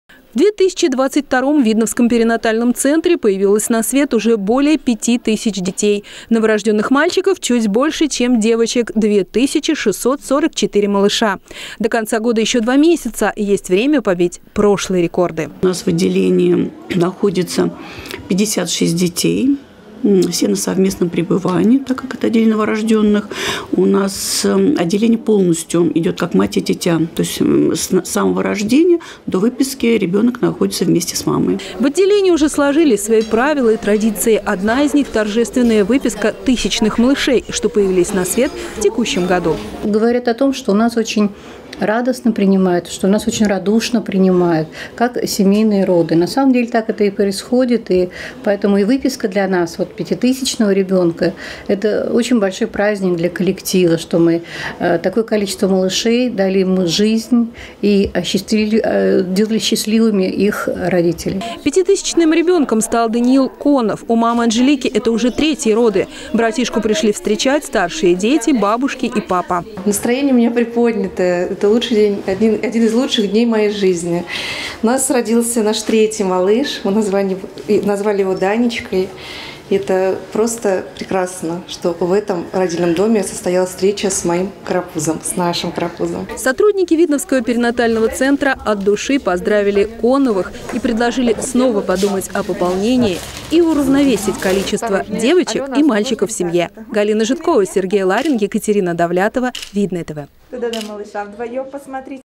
ЮБИЛЕЙНАЯ ВЫПИСКА
В Видновском перинатальном центре – очередная юбилейная выписка. Врачи и медсёстры проводили домой пятитысячного, рождённого в центре, ребёнка. С мамой новорождённого поговорила наш корреспондент Галина Жидкова.